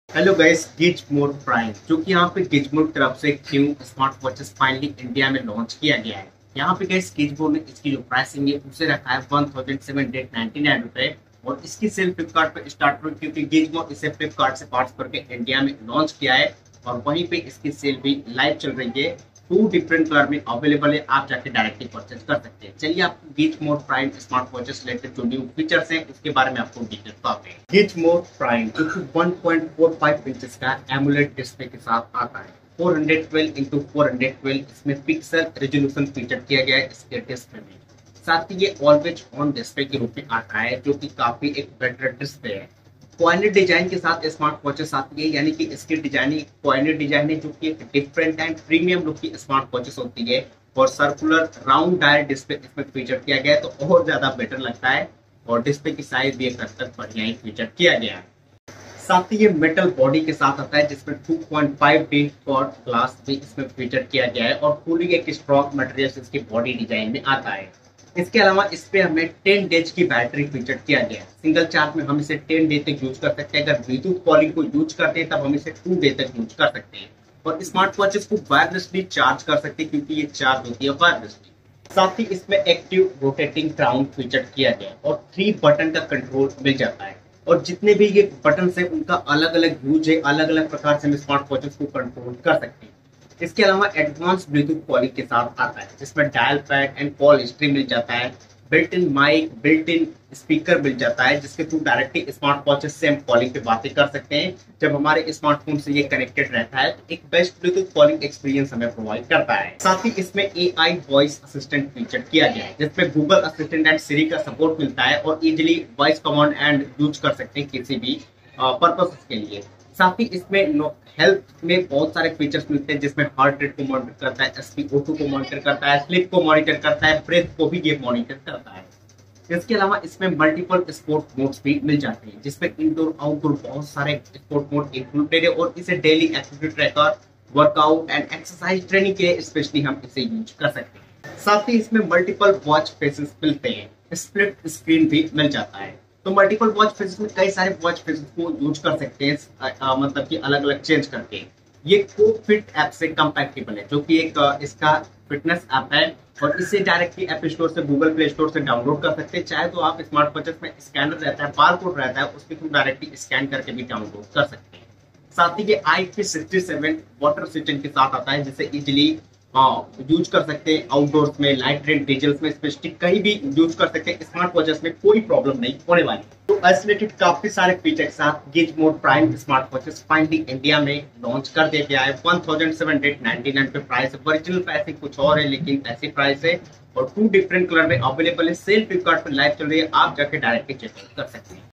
Gizmore Prime Watch | Features & Specs | Price At Rs.1799 | 1.45"Display, IP67 Water Resistance
Hai Guys Es Video Me Hm Apko Batayenge Ki Gizmore Prime Watch India Me Launch Ho Gyi Hai Aur Esase Related New Features & Price Bhi Aa Gye Hai Aur Sale Bhi Starts Ho Gyi Hai...

🎤 My Mic

🔭 My Tripod